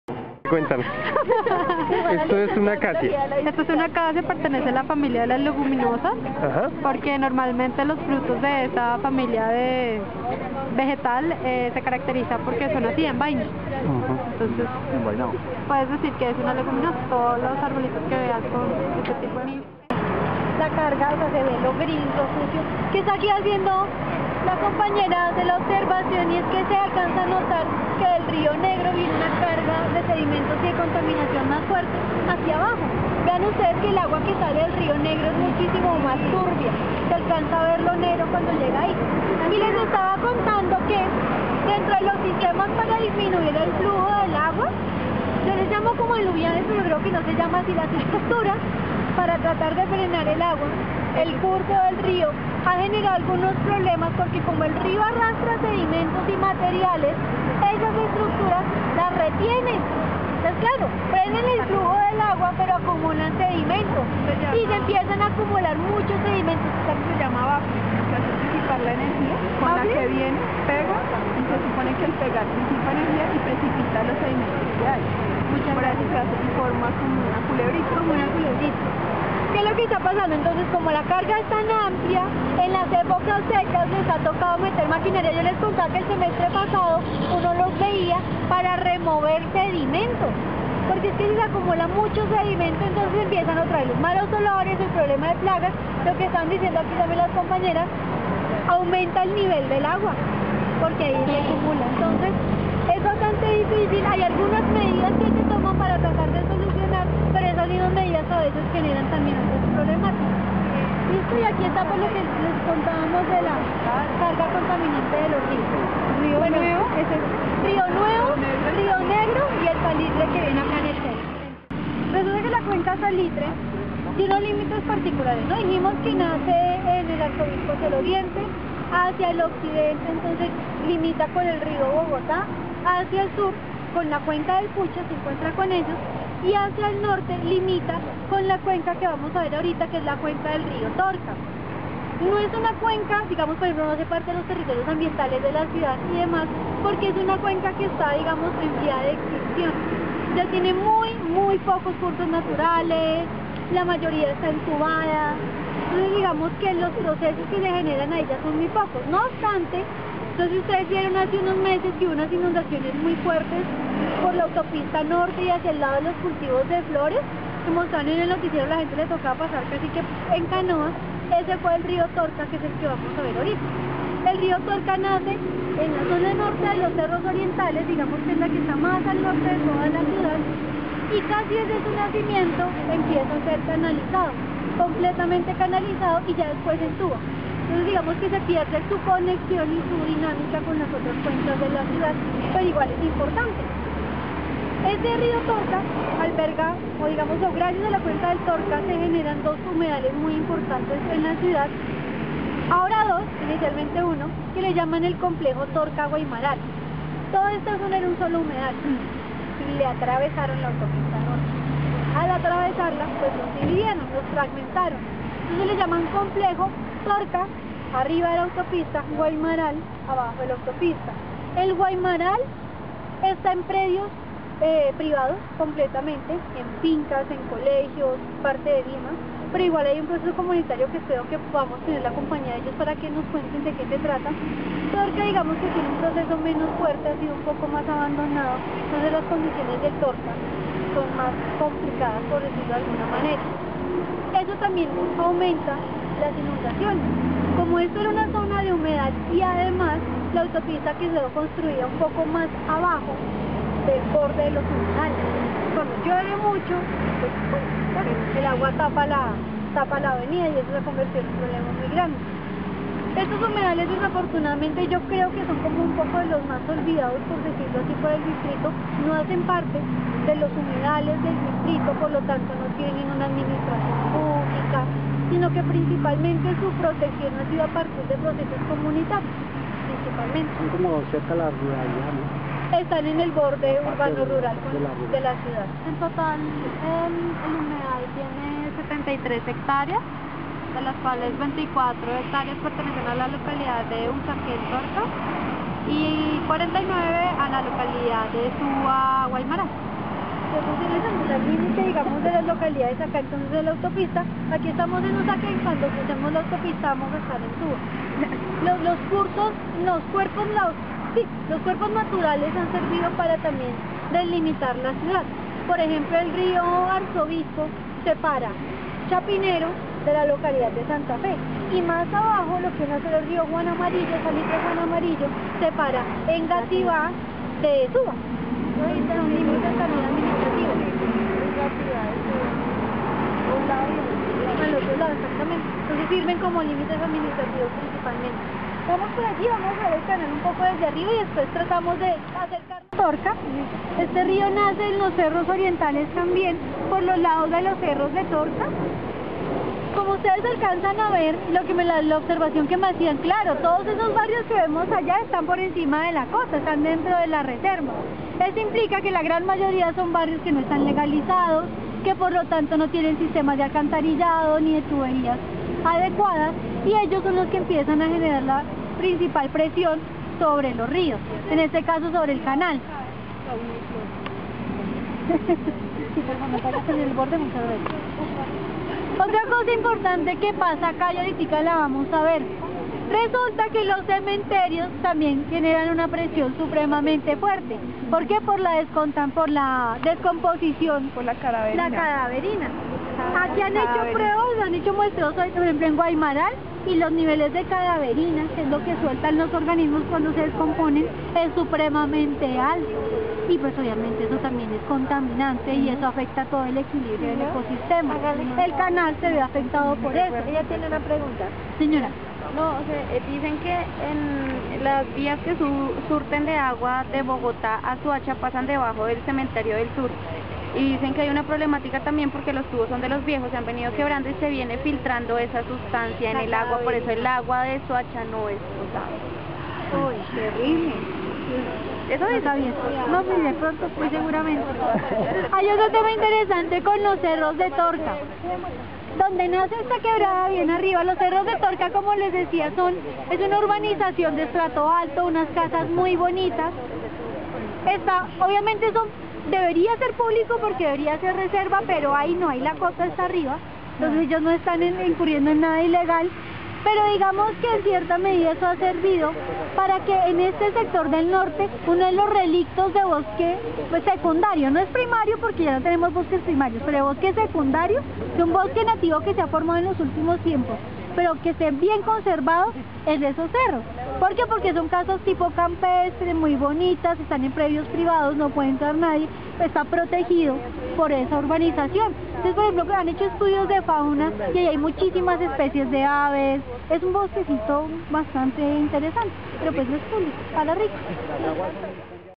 Reconocimiento Rio Arzobispo / Salitre (Parte VIII)
Aqui pueden ver la 8° parte de este recorrido facilitado por el curso para lideres ambientales del Jardin Botanico de Bogota